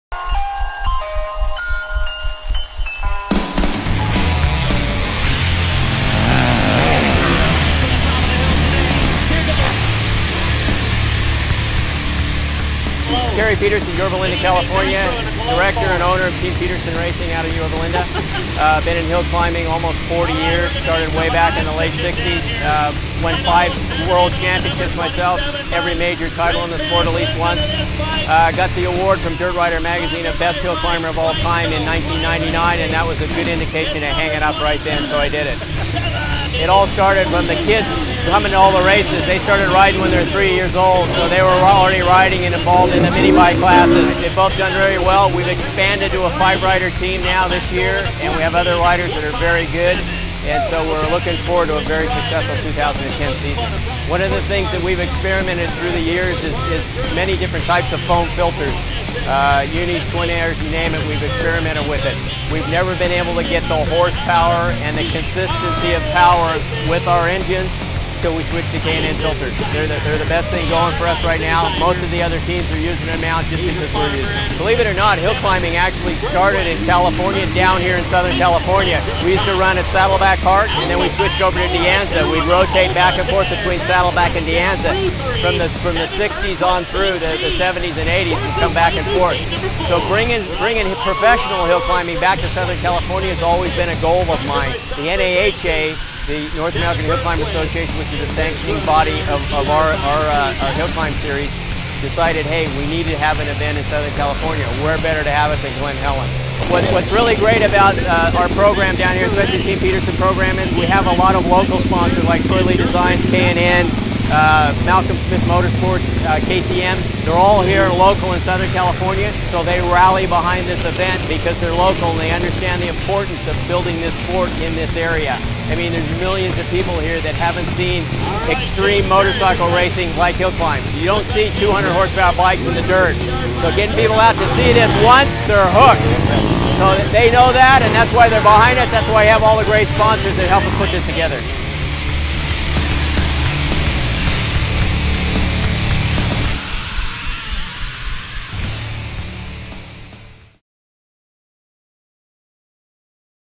NAHA World Champion Hill Climber Kerry Peterson
The NAHA (North American Hill climbers Association) is an organization of professional hill climbers and events held primarily in the western United States. World Champion Kerry Peterson discusses his involvement with Peterson Racing, the sport of hill climbing and the equipment that continues to drive his riders to the top.